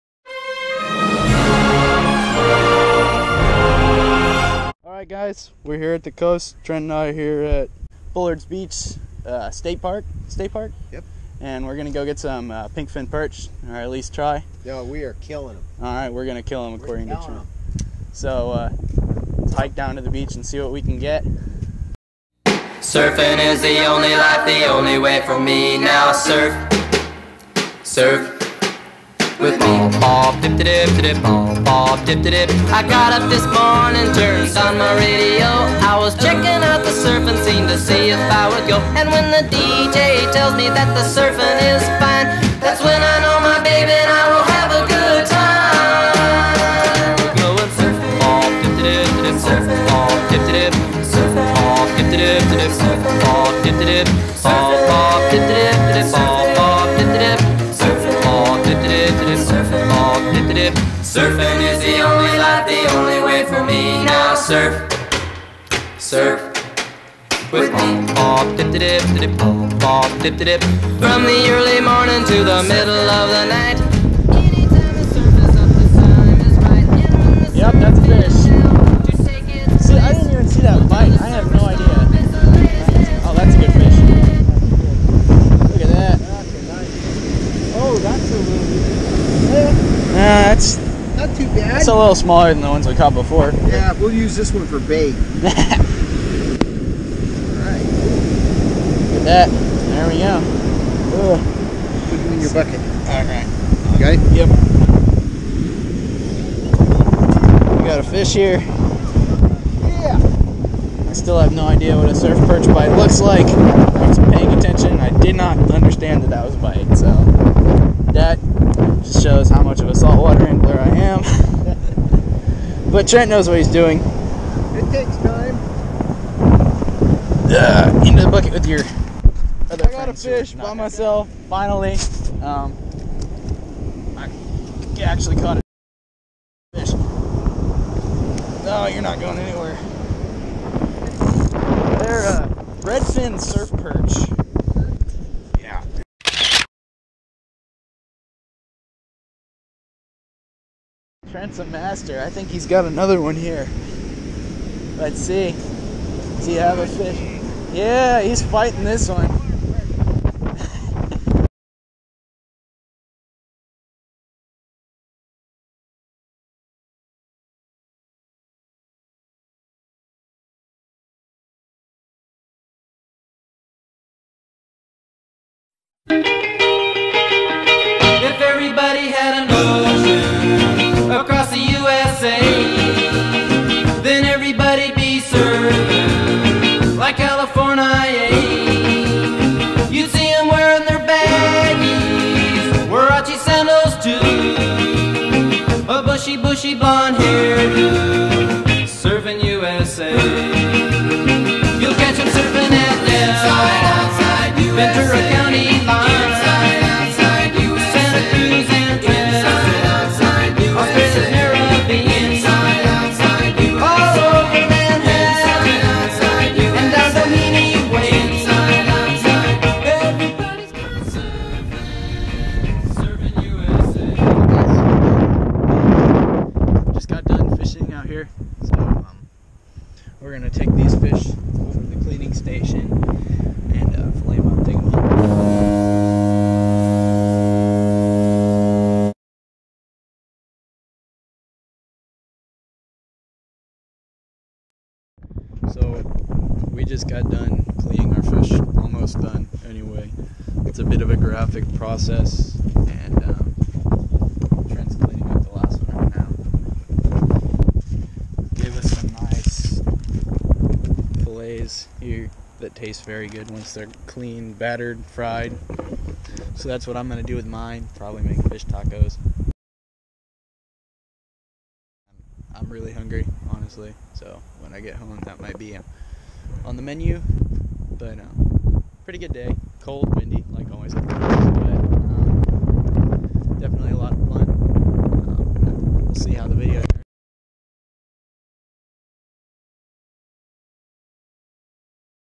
Surf Perch fishing/ mentoring project at Bullards beach
Super fun trip with Trent Drake, he taught me how to fish for those suckers and I cooked up some tasty fish tacos